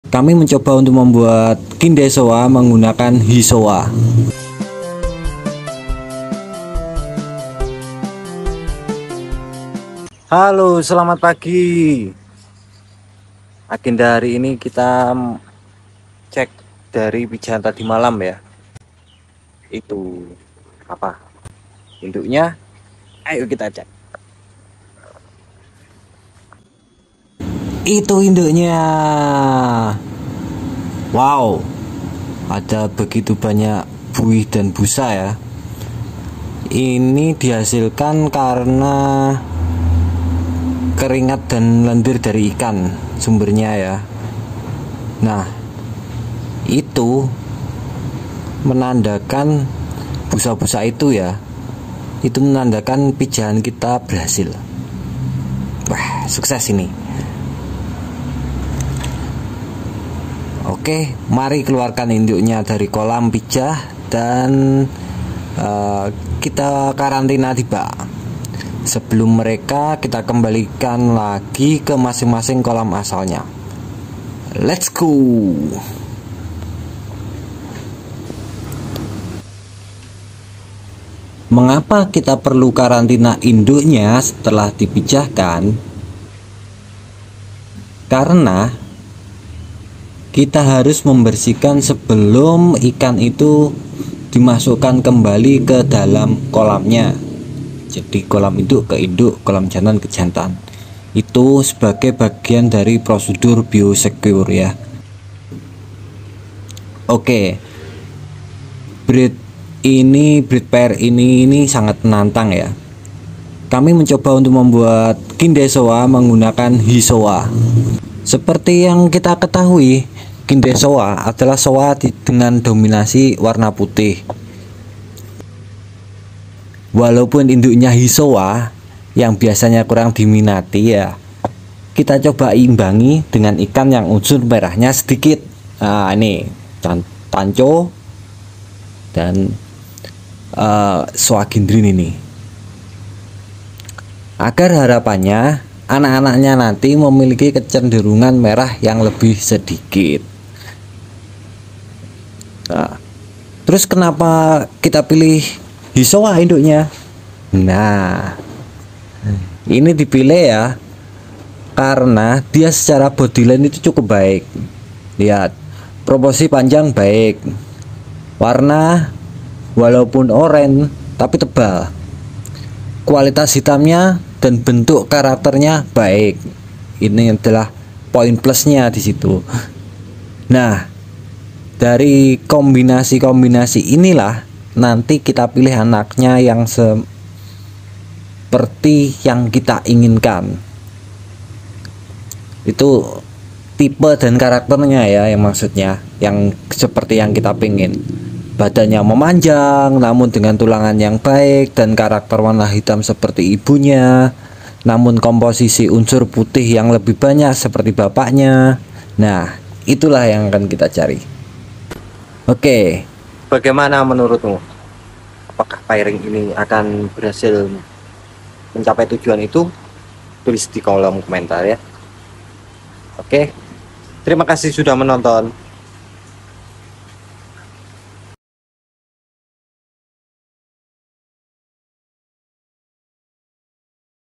MENCETAK KINDAI SHOWA DENGAN HI SHOWA
Video ini Berisi :
00:00 Intro
00:10 Greeting
00:26 Cek di kolam Pijah
00:40 Berbusa setelah Pijah
01:20 Kenapa dikarantina
01:45 Tujuan Pairing ini
02:35 Kenapa mengunakan Hi Showa
03:00 Visi perkembangan ke depan
 03:34 Closing

Kami mencoba untuk membuat kindai showa menggunakan hisowa [Musik] Halo selamat pagi agenda hari ini kita cek dari pijah tadi malam ya itu apa induknya Ayo kita cek

itu induknya

Wow ada begitu banyak buih dan busa ya ini dihasilkan karena keringat dan lendir dari ikan sumbernya Ya nah itu menandakan busa-busa itu ya itu menandakan pijah kita berhasil sukses ini

Oke mari keluarkan induknya dari kolam pijah dan kita karantina di bak sebelum mereka kita kembalikan lagi ke masing-masing kolam asalnya let's go

Mengapa kita perlu karantina induknya setelah dipijahkan

karena Kita harus membersihkan sebelum ikan itu dimasukkan kembali ke dalam kolamnya jadi koi induk ke induk koi jantan ke jantan itu sebagai bagian dari prosedur biosecure

oke breed ini breed pair ini sangat menantang ya kami mencoba untuk membuat kindai showa menggunakan Hi showa seperti yang kita ketahui kindai showa adalah Showa dengan Dominasi warna putih

walaupun induknya Hi  showa yang biasanya kurang diminati ya kita coba imbangi dengan ikan yang unsur merahnya sedikit ini Tancho

dan showa ginrin ini agar harapannya anak-anaknya nanti memiliki kecenderungan merah yang lebih sedikit

Terus kenapa kita pilih Hi showa induknya nah ini dipilih ya karena dia secara bodyline itu cukup baik lihat promosi panjang baik warna walaupun orange tapi tebal kualitas hitamnya dan bentuk karakternya baik ini adalah poin plusnya di situ Nah dari kombinasi-kombinasi inilah nanti kita pilih anaknya yang seperti yang kita inginkan

itu tipe dan karakternya ya yang Maksudnya yang seperti yang kita pingin badannya memanjang namun dengan tulangan yang baik dan karakter warna hitam seperti ibunya namun komposisi unsur putih yang lebih banyak seperti bapaknya Nah itulah yang akan kita cari Oke Bagaimana menurutmu Apakah pairing ini akan berhasil mencapai tujuan itu tulis di kolom komentar ya Oke terima kasih sudah menonton